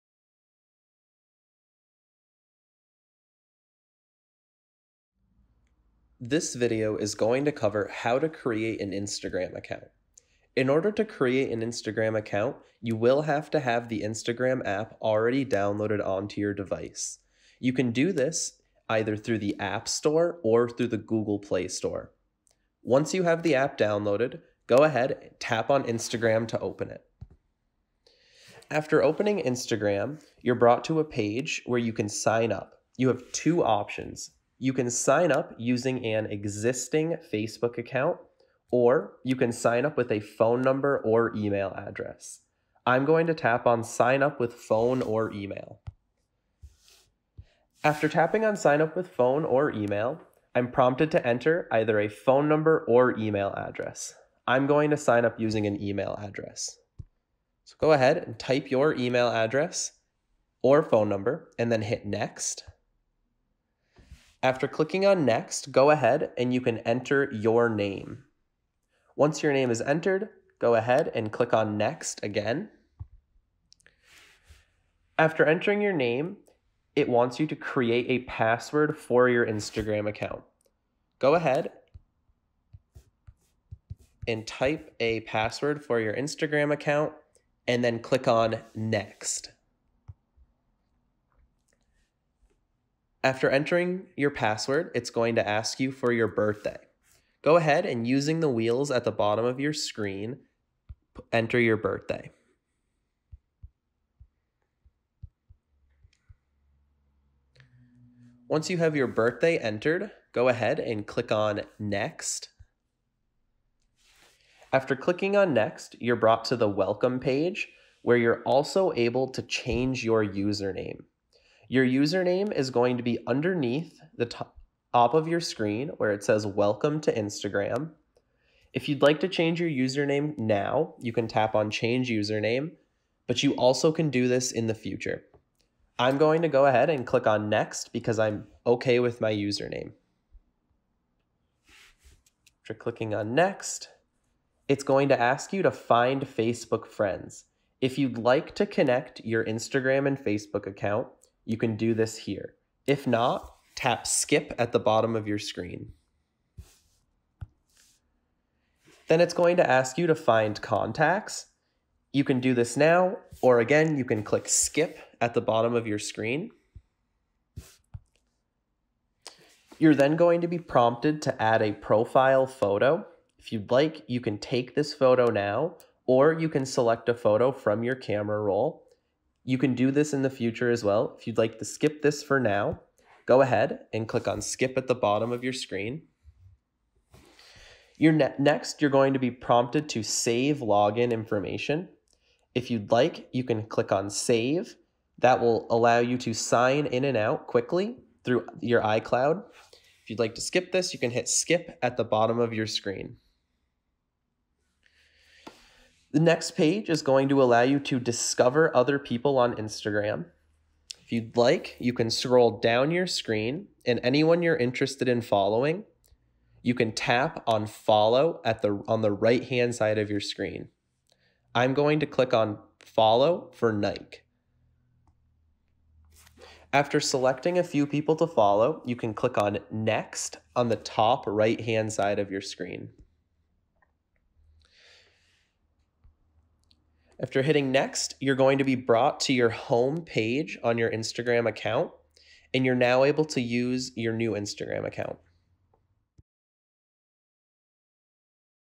How to Create an Instagram Account
Learn how to set up a personal account on the popular social media platform Instagram.
Discover more tech tips at seniorplanet.org